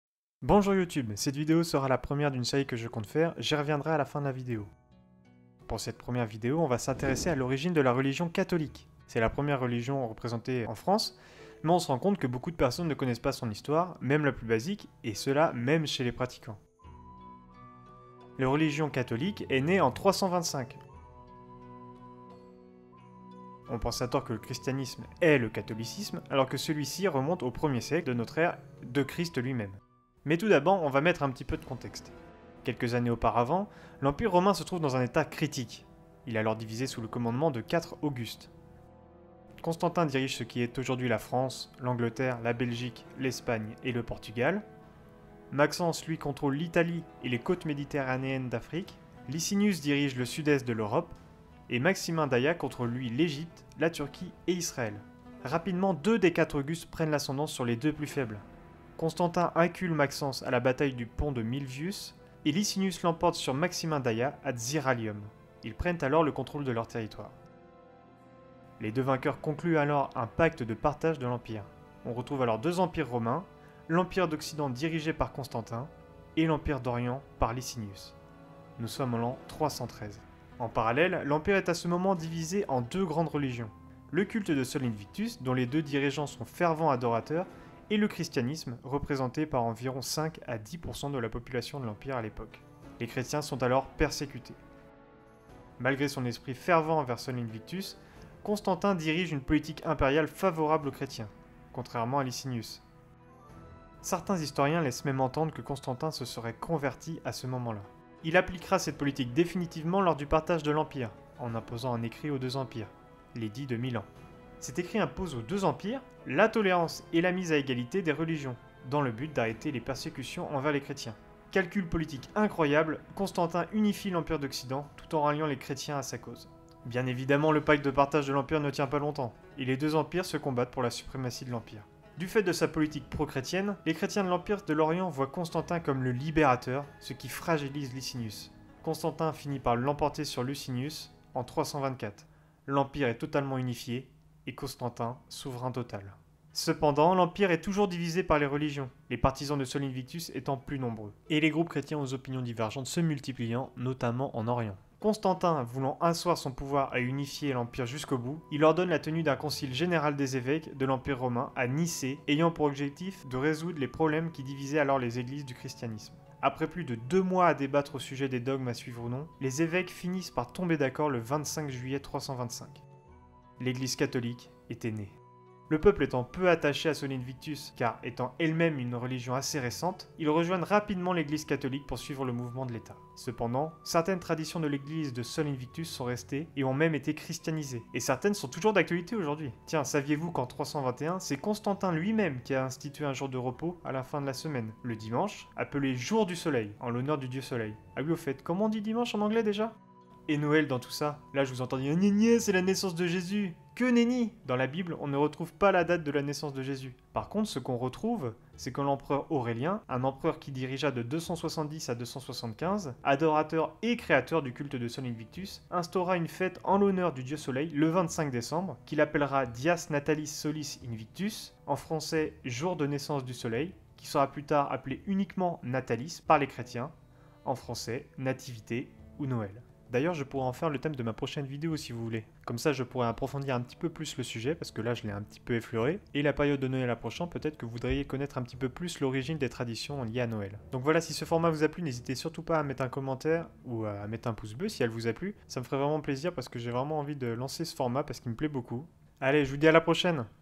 LES ORIGINES #1 - LE CATHOLICISME
Bonjour Youtube !

Aujourd'hui je lance un nouveau format de vidéo dans lequel je vais expliquer l'origine des choses, et pour cette première vidéo je vais expliquer l'origine du catholicisme.

N'hésitez pas à me faire vos retours en commentaire.